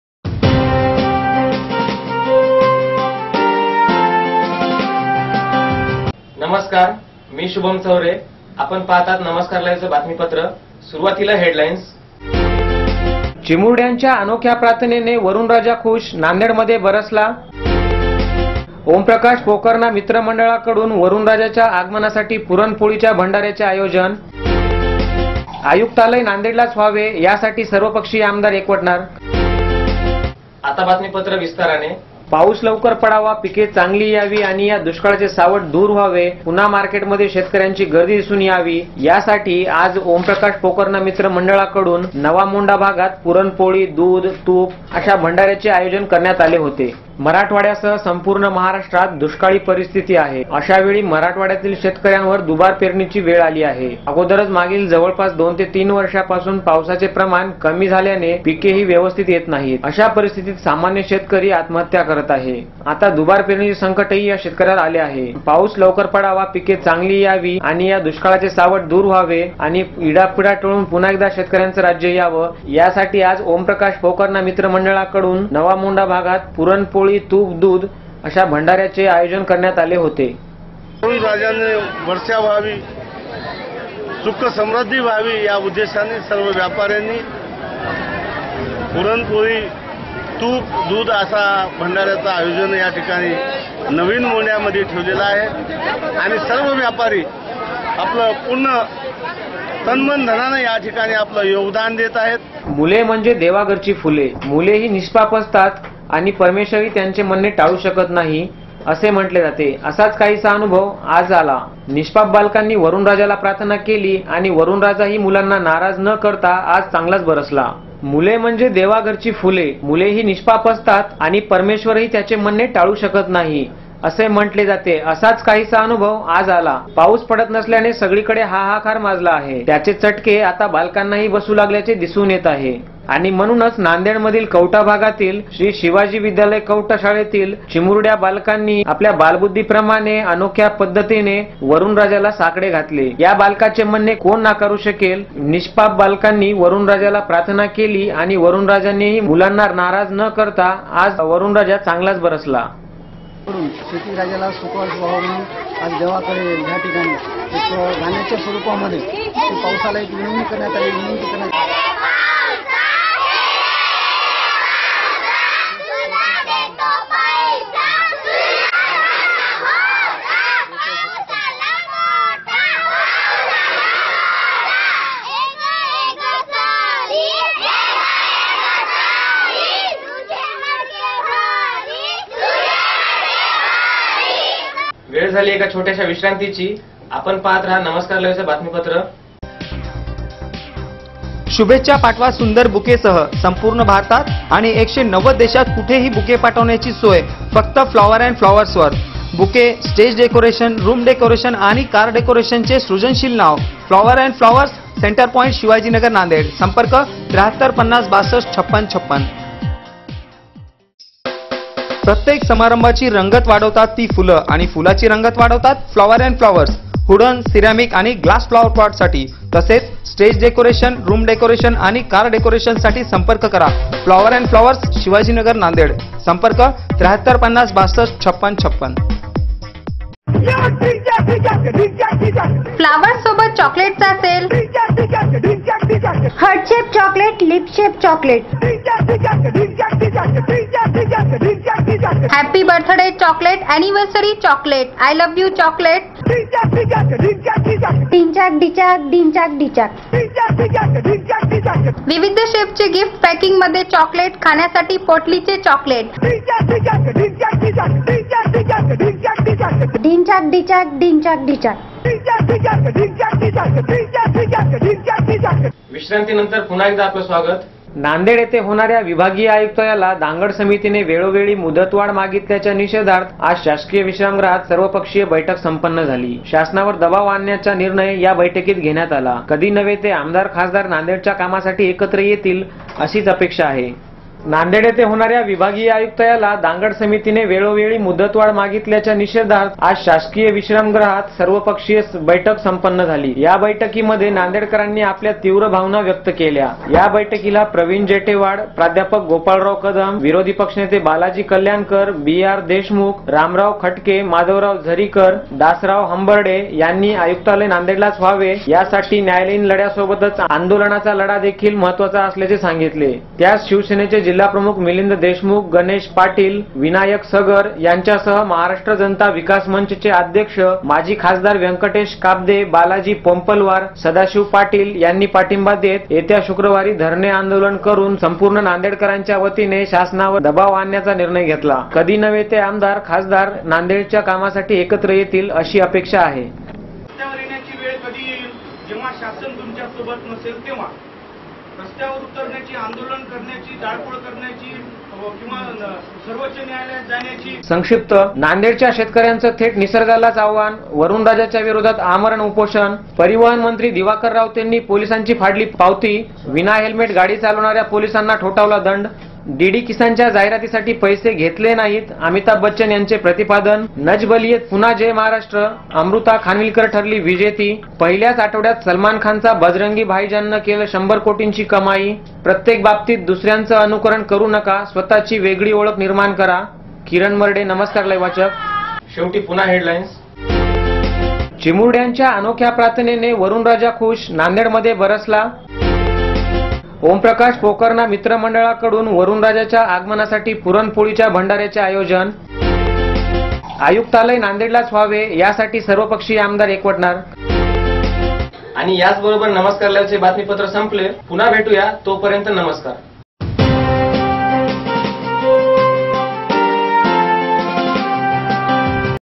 Namaskar Live 20-07-2015 Video News
नमस्कार लाईव्ह दि.२०/०७/२०१५ व्हिडीओ बातमीपत्र
१) चिमुरड्यांच्या अनोख्या प्रार्थनेमुळे वरूणराजा खुश, नांदेडमध्ये बरसला.
२) ओमप्रकाश पोखर्णा मित्र मंडळाकडून वरूणराजाच्या आगमनासाठी पुरण - पोळीच्या भंडाऱ्याचे आयोजन
३) आयुक्तालय नांदेडलाच व्हावे यासाठी सर्व पक्षीय आमदार एकवटणार.
संक्षिप्त
१) नांदेडच्या शेतकऱ्याचं थेट निसर्गालाच आव्हान, वरुणराजाविरोधात आमरण उपोषण.
२) परिवहन मंत्री दिवाकर रावतेंनी पोलिसांची फाडली पावती, विनाहेल्मेट गाडी चालवणाऱ्या पोलिसांना ठोठावला दंड.
३) 'डीडी किसान'च्या जाहिरातीसाठी पैसे घेतले नाहीत: अमिताभ बच्चन यांचे प्रतिपादन .
४) ‘नच बलिए’त पुन्हा ‘जय महाराष्ट्र’, अमृता खानविलकर ठरली विजेती.
५) पहिल्याच आठवड्यात सलमान खानच्या  'बजरंगी भाईजान'नं केली १०० कोटींची कमाई.
सुविचार
प्रत्येक बाबतीत दुसऱ्याच अनुकरण करु नका; स्वतःची वेगळी ओळख निर्माण करा
{ सतीश कदम, नमस्कार लाईव्ह वाचक }
नमस्कार लाईव्ह २०-०७-२०१५ सायंकाळचे बातमीपत्र

१- १० जुलै पासून चिक्कीचे वाटप पूर्णपणे थांबविले, सरकारची हायकोर्टात कबुली
२- पिक विम्याच्या नुकसान भरपाईची रक्कम थेट शेतकऱ्यांच्या बचत खात्यात जमा करा - पालकमंत्री दिवाकर रावते
३- किनवट तालुक्यात मौजे मदनापुर येथे नांदेड राष्ट्रीय कृषी ग्रामीण विकास संस्थेच्या वतीने वित्तीय साक्षरता जागरुकता अभियान संपन्न
४- सोन्याच्या दरात मोठी घसरण, पाच वर्षात पहिल्यांदाच सोनं २५ हजाराखाली
५- देगलूरमध्ये बँकेकडून शेतकऱ्यांवर अन्याय